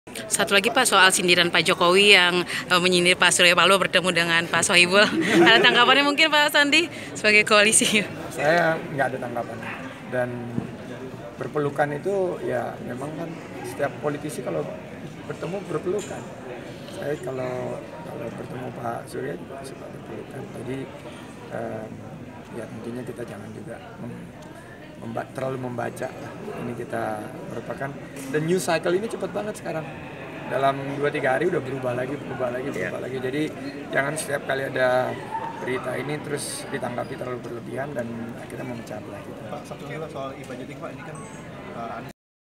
Jokowi Sindir Surya Paloh Rangkul Sohibul Iman, Sandiaga: Saya Suka Berpelukan
Eks Cawapres Sandiaga Uno enggan mengomentari secara berlebihan soal sindiran Presiden Jokowi terkait pertemuan Ketua Umum Partai Nasdem Surya Paloh dengan Presiden PKS Sohibul Iman.